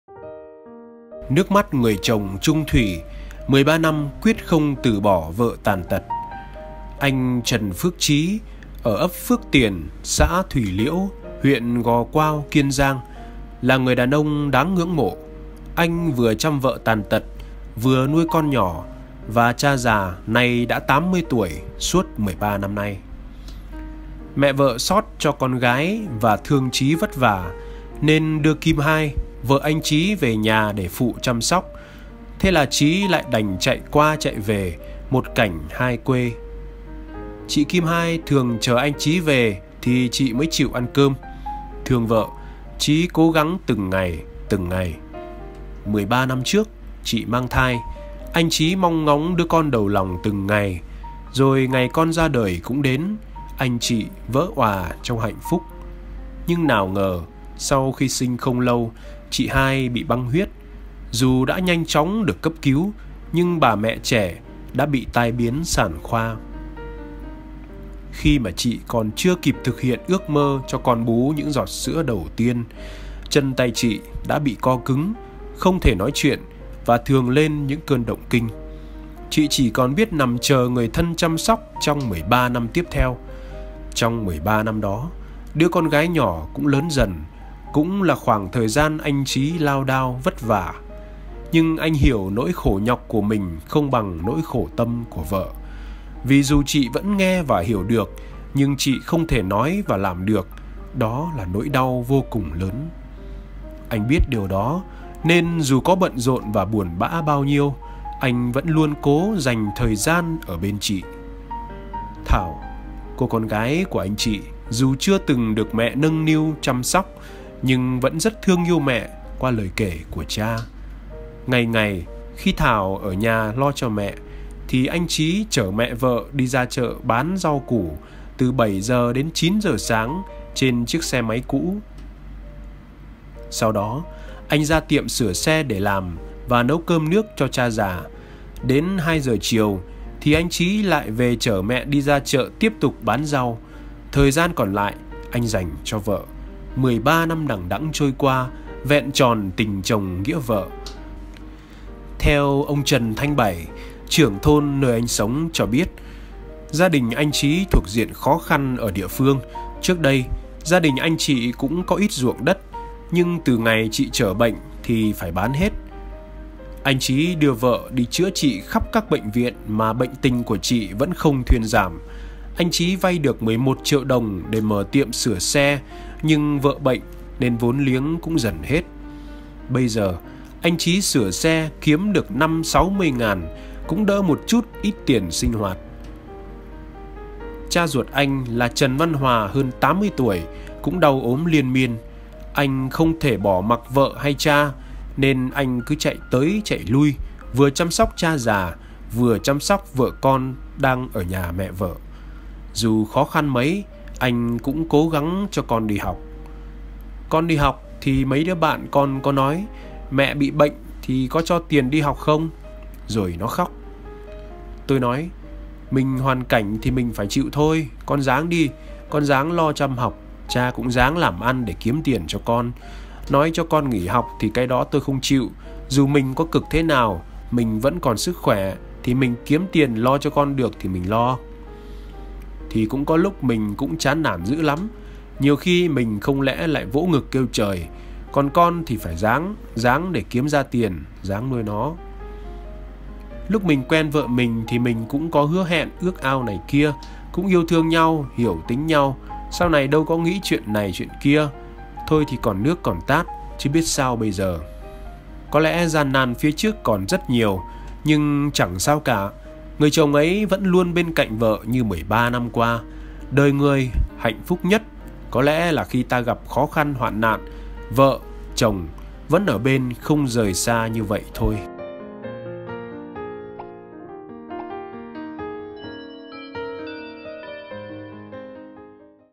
13 năm QUYẾT KHÔNG TỪ BỎ vợ tàn tật
Anh Trần Phước Trí ở ấp Phước Tiền, xã Thủy Liễu, huyện Gò Quao, Kiên Giang là người đàn ông đáng ngưỡng mộ. Vừa chăm con nhỏ, vừa nuôi cha già 80 tuổi, cuộc sống tuy nhiều khó khăn nhưng anh vẫn không bỏ rơi người vợ tàn tật của mình.
Những chia sẻ của anh thật cảm động. Xin mời quý bạn cùng xem video: "13 năm quyết không từ bỏ vợ tàn tật và nước mắt của người chồng chung thuỷ".